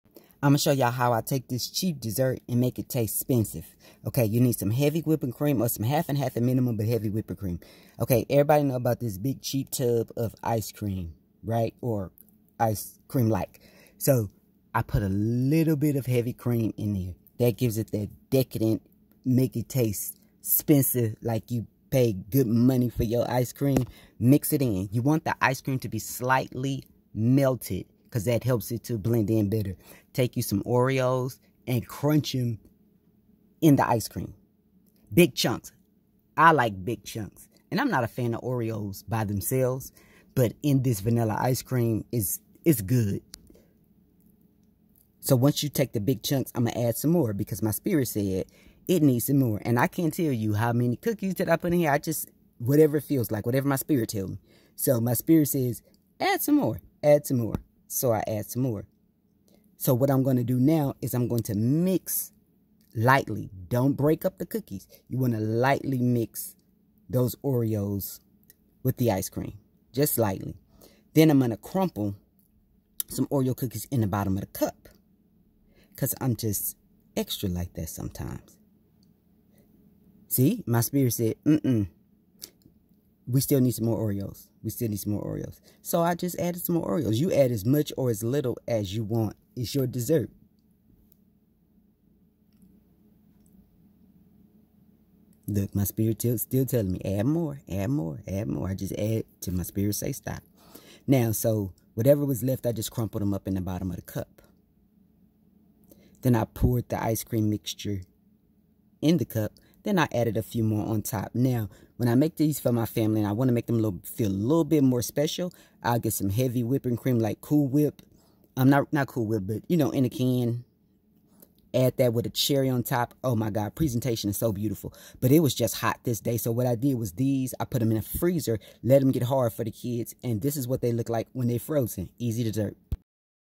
CHEAP AND EASY DESSERT.
This is how I make a cheap and easy dessert. It's s delicious.

🍦  🍨  ice cream. dessert.quick and easy. cheap.